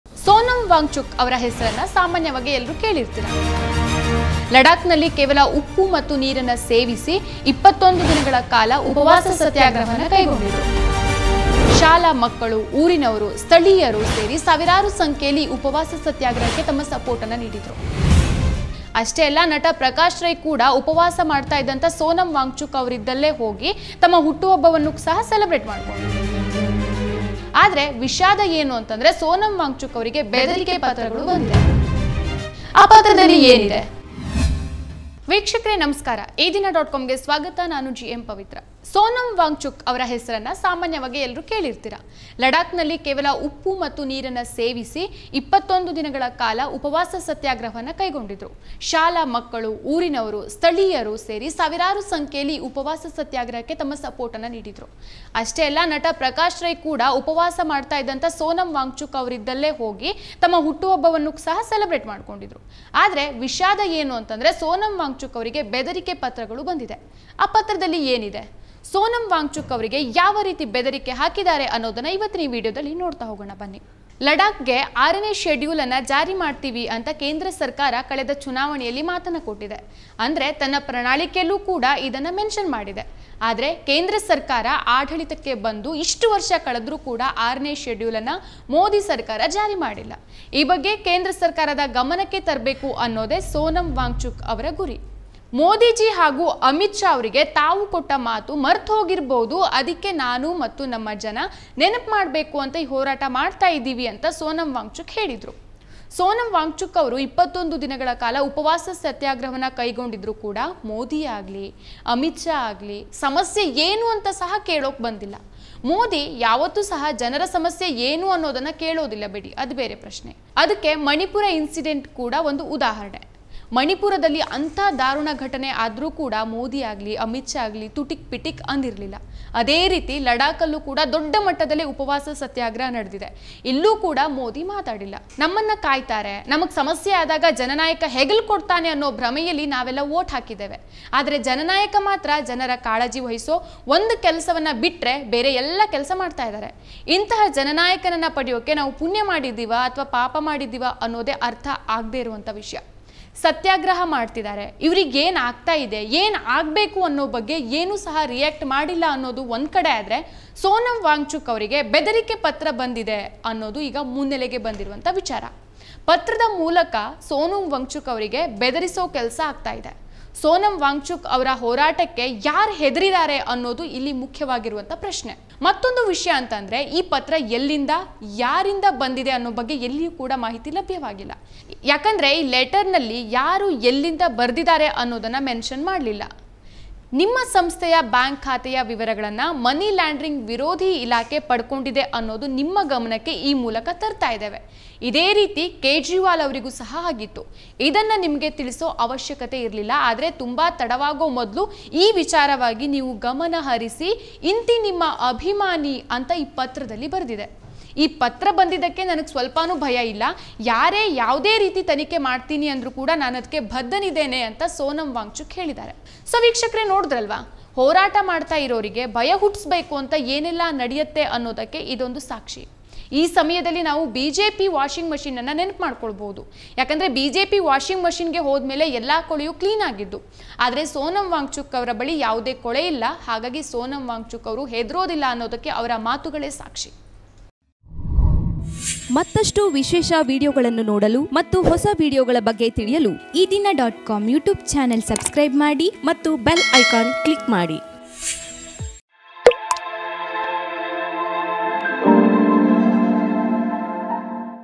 ಭರವಸೆಗಳನ್ನು ಮುರಿಯೋದು ಅಪರಾಧವಲ್ಲ ಆದರೆ ಪ್ರಶ್ನೆಗಳನ್ನು ಕೇಳುವುದೇ ಮಹಾಪರಾಧವೇ ?
ಸೋನಮ್ ವಾಂಗ್ಚುಕ್ ಅವರ ಹೆಸರನ್ನು ಸಾಮಾನ್ಯವಾಗಿ ಎಲ್ಲರೂ ಕೇಳಿರ್ತೀರಾ. ಲಡಾಕ್ನಲ್ಲಿ ಕೇವಲ ಉಪ್ಪು ಮತ್ತು ನೀರನ್ನು ಮಾತ್ರ ಸೇವಿಸಿ 21 ದಿನಗಳ ಕಾಲ ಉಪವಾಸ ಸತ್ಯಗ್ರಹವನ್ನ ಕೈಗೊಂಡಿದ್ರು. ಶಾಲಾ ಮಕ್ಕಳು, ಊರಿನವರು, ಅಲ್ಲಿನ ಸ್ಥಳೀಯರು ಸೇರಿ ಸಾವಿರಾರು ಸಂಖ್ಯೆಯಲ್ಲಿ ಈ ಉಪವಾಸ ಸತ್ಯಾಗ್ರಹಕ್ಕೆ ತಮ್ಮ ಸಪೋರ್ಟ್ ನೀಡಿದ್ರು.. ಅಷ್ಟೇ ಅಲ್ಲ ನಟ ಪ್ರಕಾಶ್ ರೈ ಕೂಡ ಉಪವಾಸ ಮಾಡ್ತಾ ಇದ್ದ ಸೋನಮ್ ವಾಂಗ್ಚುಕ್ ಅವರಿದ್ದಲ್ಲೇ ಹೋಗಿ ತಮ್ಮ ಹುಟ್ಟು ಹಬ್ಬವನ್ನೂ ಕೂಡ ಸೆಲೆಬ್ರೇಟ್ ಮಾಡಿಕೊಂಡಿದ್ರು.. ಆದ್ರೆ ವಿಷಾದ ಅಂದ್ರೆ ಸೋನಮ್ ವಾಂಗ್ಲುಕ್ ಅವರಿಗೆ ಬೆದರಿಕೆ ಪತ್ರಗಳು ಬಂದಿದೆ.. ಆ ಪತ್ರದಲ್ಲಿ ಏನಿದೆ ? ಸೋನಮ್ ಅವರಿಗೆ ಯಾವ ರೀತಿ ಬೆದರಿಕೆ ಹಾಕಿದ್ದಾರೆ ಅನ್ನೋದನ್ನ ಈ ವಿಡಿಯೋದಲ್ಲಿ ನೋಡೋಣ...